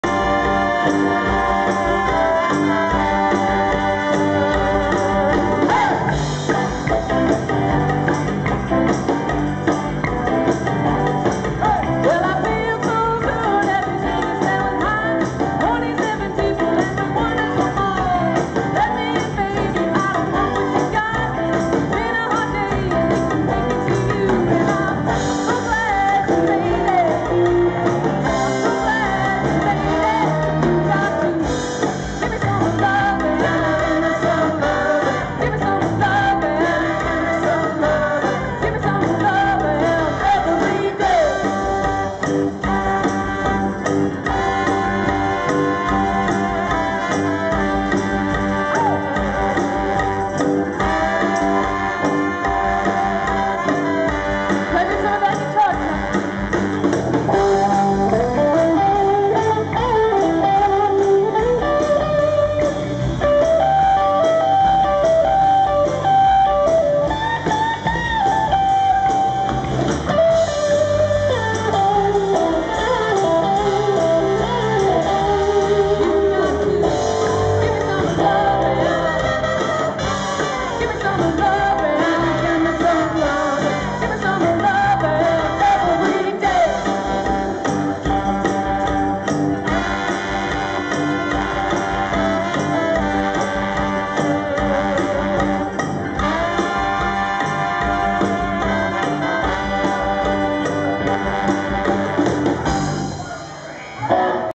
The Mid-Life Crisis Band at The Pink Galleon
The Mid-Life Crisis Band at The Pink Galleon in the Town Plaza, Cape Girardeau, MO